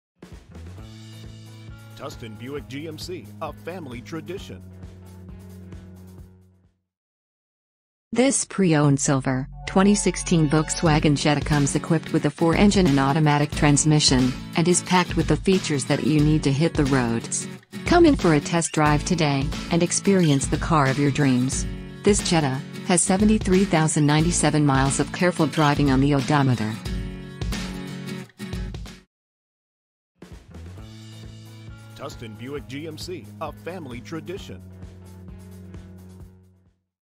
USED 2016 VOLKSWAGEN JETTA 1.4T S at Tustin Buick GMC (USED)
This is a USED 2016 VOLKSWAGEN JETTA 1.4T S offered in Tustin California by Tustin Buick GMC (USED) located at 1 Auto Center Drive, Tustin, California

Stock Number: 5117T